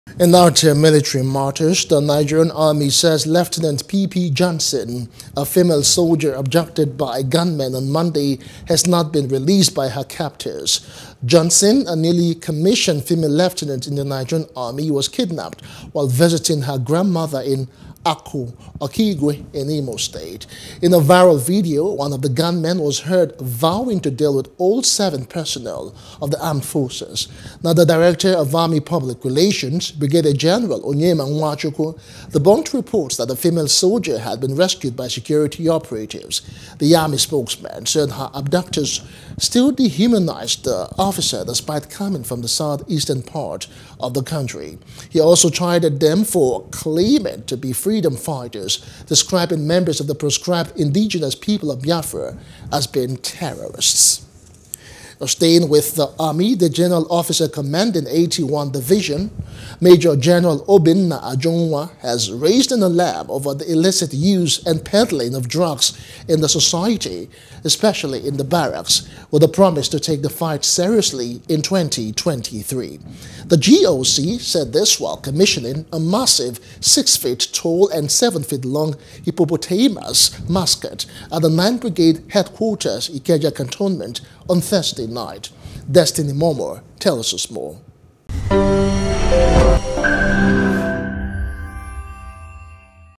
Abduction: Female Soldier Yet To Be Released – Army
The Nigerian Army SAYS Lieutenant PP Johnson, a female soldier abducted by gunmen on Monday, has not been released by her captors.